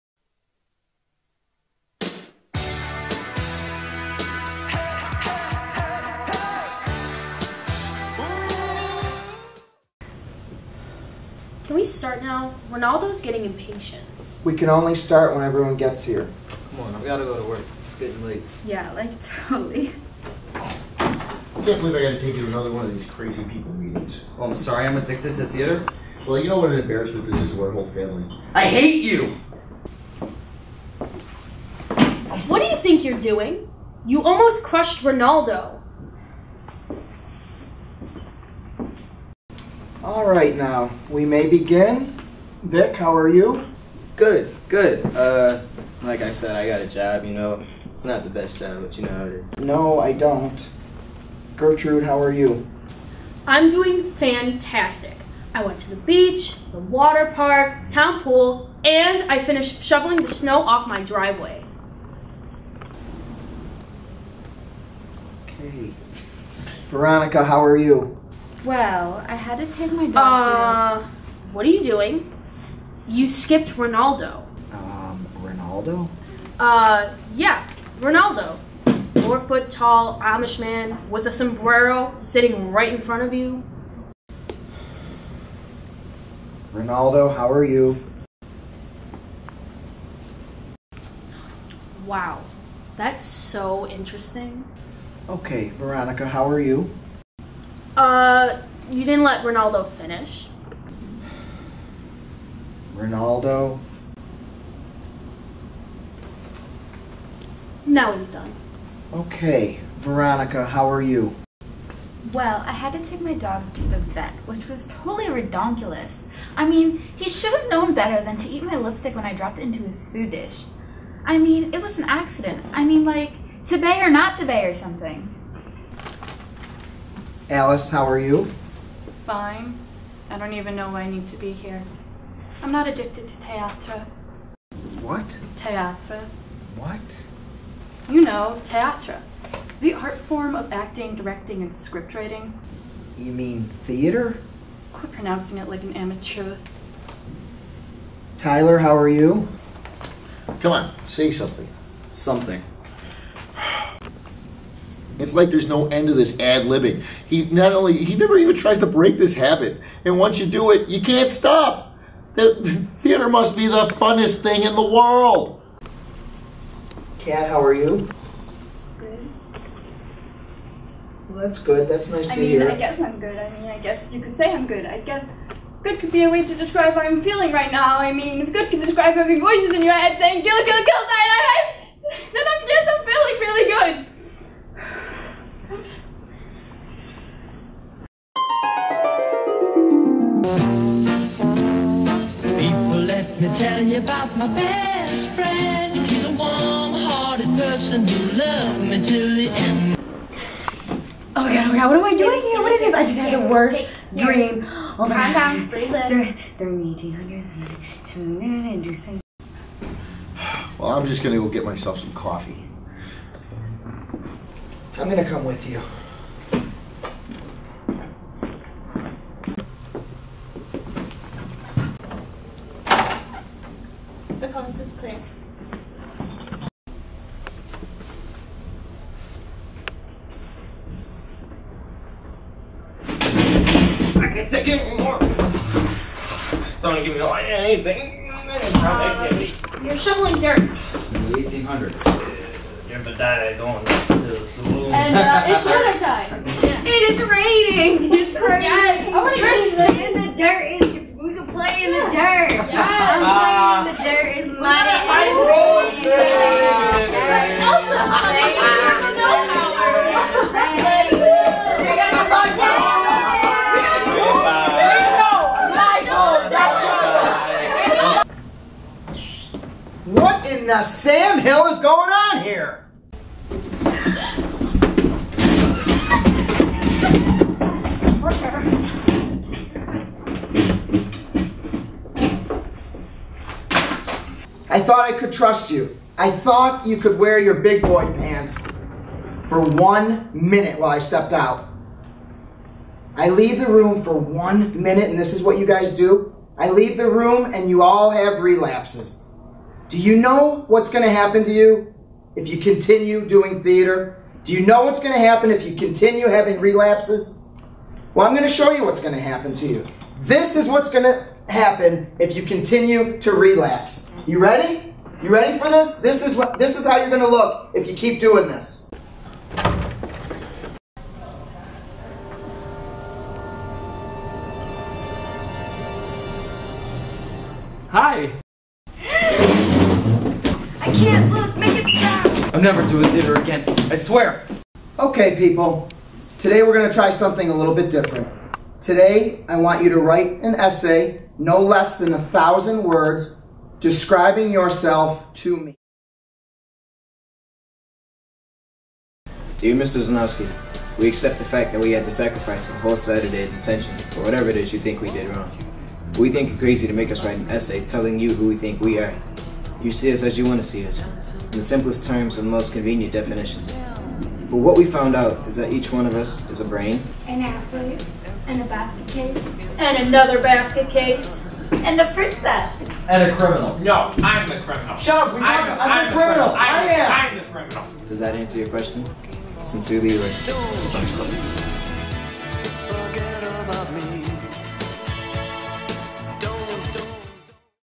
The Brunch Club
Instead of detention, they're in an AA Meeting. A parody of the famous film "The Breakfast Club" starring theater kids from Kenmore East High School, who end up in an AA Meeting (Actors Anonymous) for being addicted to Theater.

Thanks to everyone who participated! (In order of appearance)

Officer Z/Mr. Zenosky - Officer Z/Mr. Zenosky
Gertrude - Grace
Vic - Bryan
Veronica - Kiera
Dad - Mr. A
Tyler - Jake
Ronaldo - Ronaldo
Alice - Taylor
Kat - Katrine (We miss you!)
Girl sleeping - Haley
Best friend - Miranda
Guy going through really bad withdrawal/Duke - Ryan
Scary guy - Mr. Vogt

And of course Mrs. Hanagan for being the best teacher ever! :)